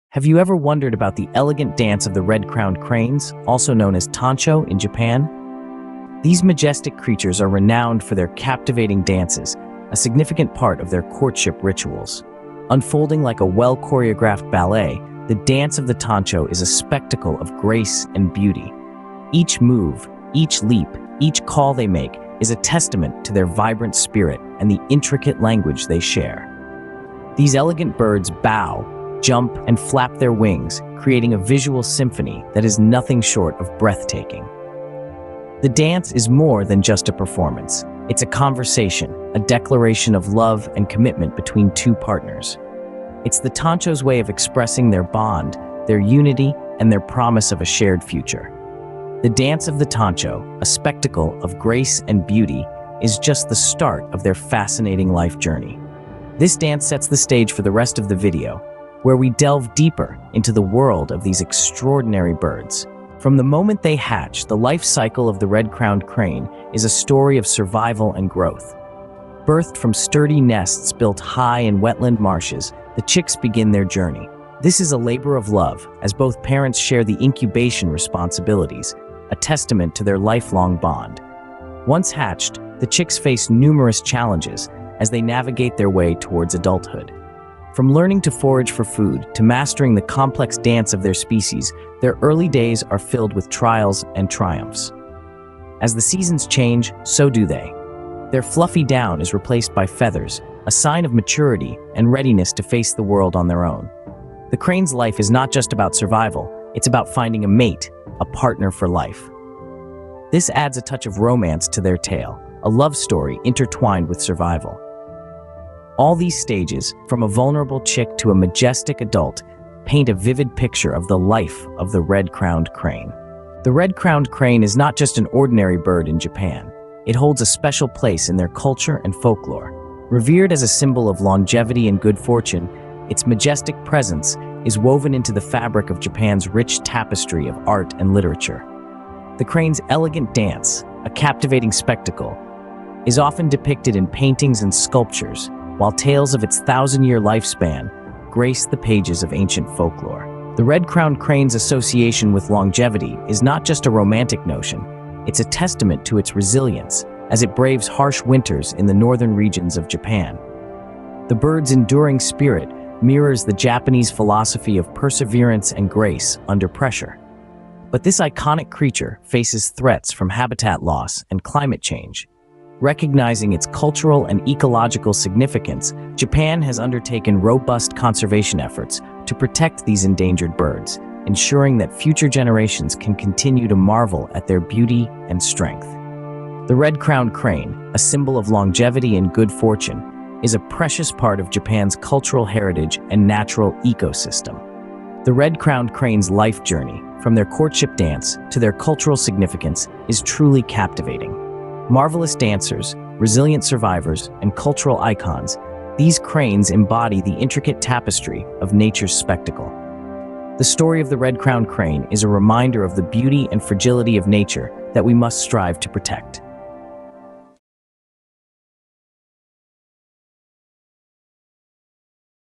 Tancho Tango: Japan's Red-Crowned Crane Spectacle
Enter the enchanting world of Japan's iconic red-crowned cranes, affectionately known as tancho, as they perform their mesmerizing dance amidst serene marshlands and snowy landscapes. Marvel at their graceful courtship rituals, where intricate movements and calls convey deep bonds of love and devotion. Explore their nesting behaviors, from the meticulous construction of nests to the tender care of their young, showcasing the dedication of these majestic birds to their families. Beyond their natural beauty, delve into the profound cultural significance of the tancho in Japanese folklore and art, where they symbolize longevity, fidelity, and good fortune. Through their elegant dance and timeless presence, the red-crowned cranes embody the enduring connection between humanity and the natural world, inspiring reverence and admiration for generations to come.